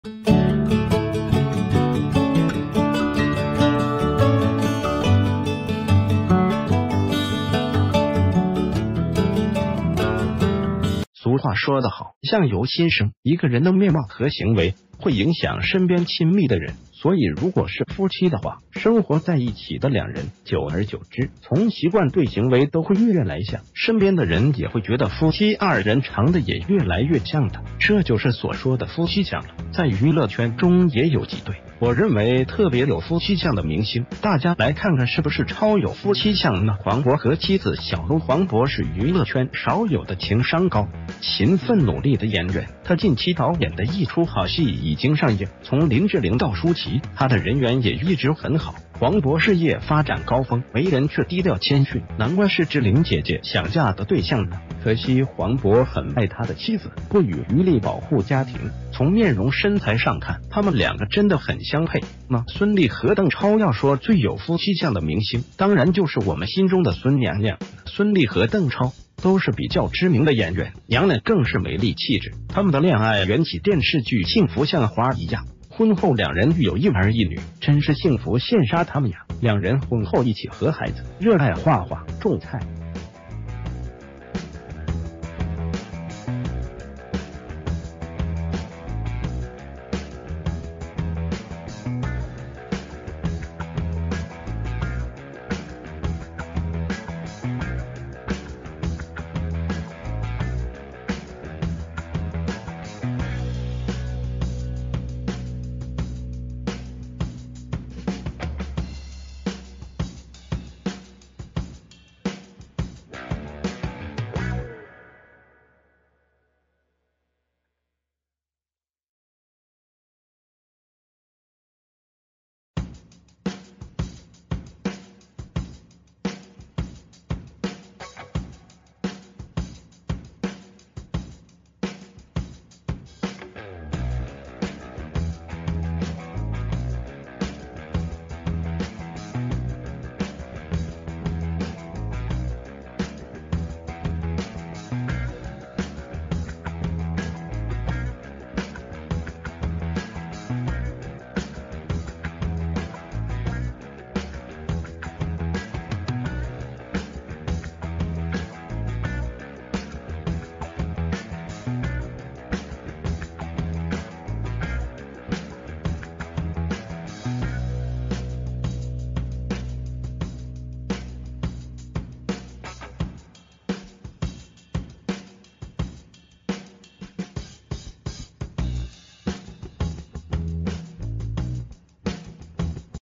最有夫妻相的明星夫妻，我心中就這2對
俗話說的好：相由心生。一個人的面貌和行為會影響身邊親密的人。
    所以如果是夫妻的話，生活在一起的兩人，久而久之，從習慣對行為都會越越來像，身邊的人也會覺得夫妻二人長的也越來越像了，這就是所說的“夫妻相”了。
    在娛樂圈中也有幾對，我認為特別有夫妻相的明星，大家來看看是不是超有夫妻相呢黃渤和妻子小歐
    黃渤是娛樂圈少有的情商高、勤奮努力的演員，他近期導演的《一出好戲》已經上映，從林志玲到舒淇，他的人緣也一直很好。黃渤事業發展高峰，為人卻低調謙遜，難怪是志玲姐姐想嫁的對象呢。可惜黃渤很愛他的妻子，不予余力保護家庭，從面容身材上看，他們兩個真的很相配呢孫儷和鄧超
    要說最有夫妻相的明星，當然就是我們心中的孫娘娘呢
    孫儷和鄧超都是比較知名的演員，娘娘更是美麗氣質。他們的戀愛源起電視劇《幸福像花兒一樣》。婚后兩人育有一兒一女，真是幸福羨煞他們呀。兩人婚后一起和孩子，熱愛畫畫，種菜，旅游，自拍。生活過成詩般經常。他們也是小編最喜歡的一對，超級有夫妻相，是不是。祝愿他們幸福永恒！